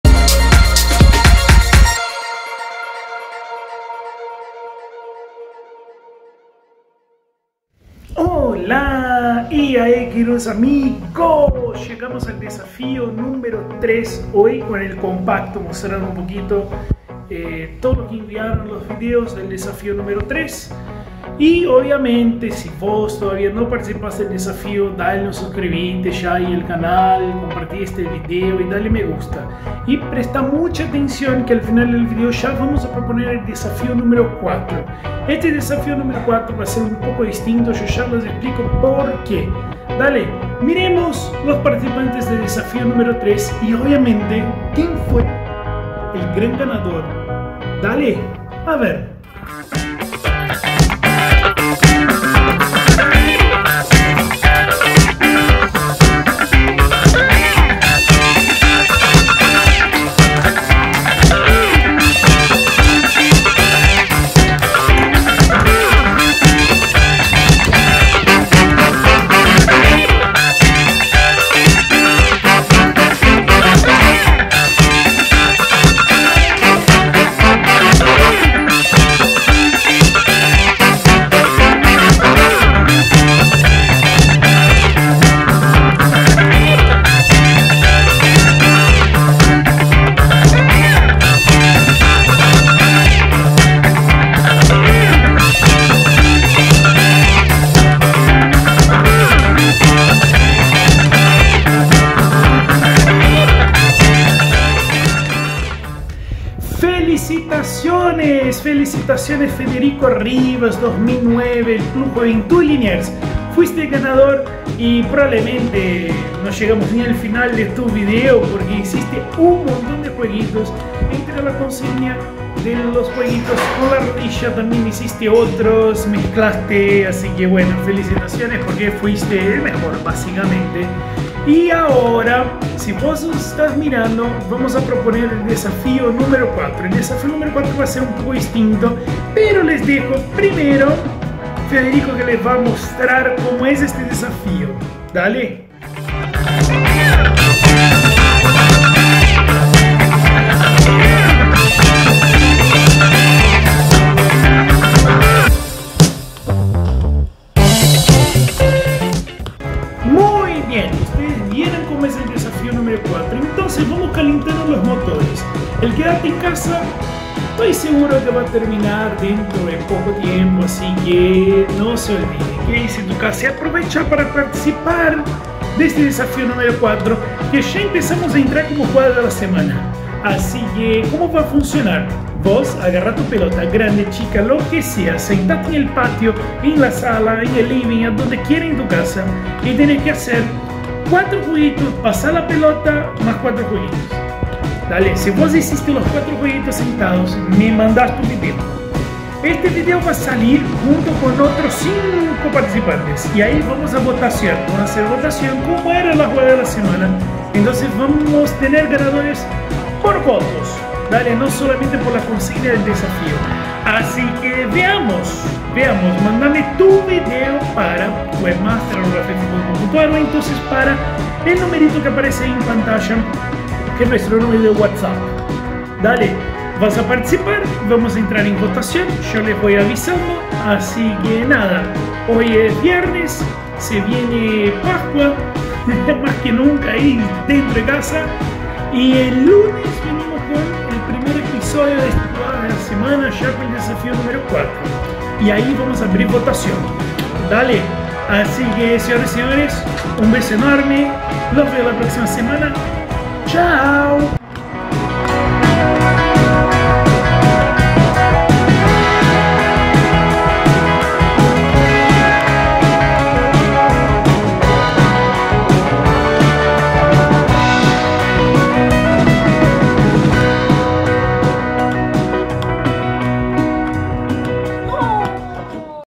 Desafío del Brasuka - Muslo Rodilla - Ep.#3
El Desafío del Br4suk4 en FEFI llegá al episodio #3.
Hemos desafiado a todos con hacer la máxima cantidad de jueguitos con muslo y rodlla. El resultado, el ganador y el nuevo desafío lo ves en este vídeo.

Para participar del desafío #4 debes enviar tu vídeo por Whatsapp al número: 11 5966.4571
Envía tu vídeo hasta el Domingo de Páscua, 12 de Abril